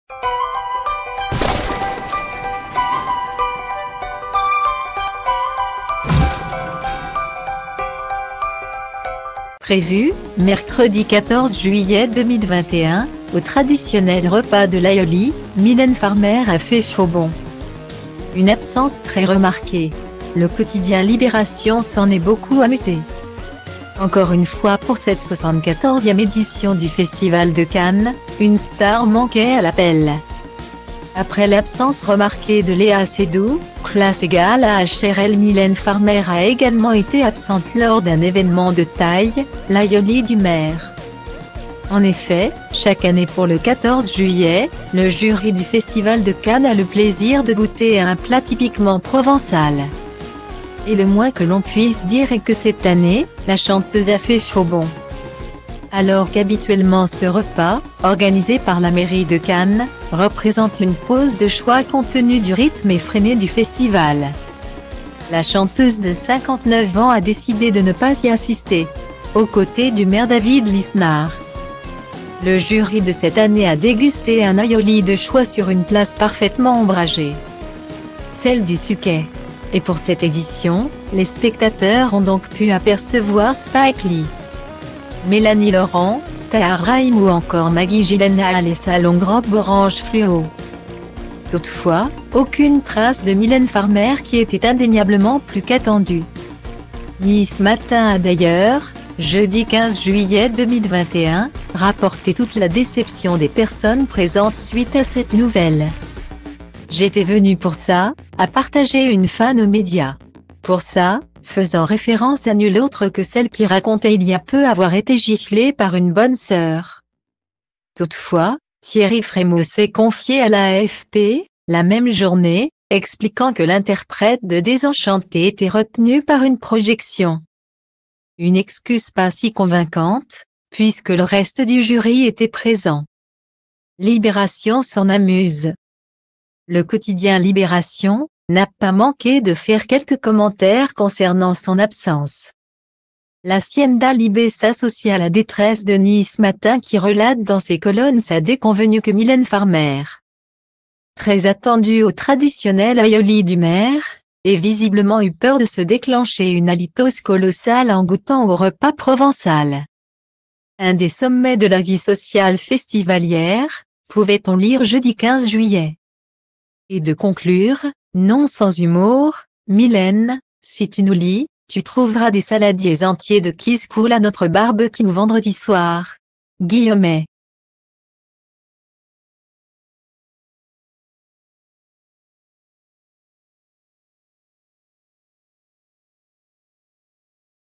Mylène Farmer snobe un événement à et se fait afficher !
Prévue, mercredi 14 juillet 2021, au traditionnel repas de l'aïoli, Mylène Farmer a fait faux bond.Une absence très remarquée. Le quotidien Libération s'en est beaucoup amusé. . . Encore une fois pour cette 74ème édition du Festival de Cannes, une star manquait à l'appel.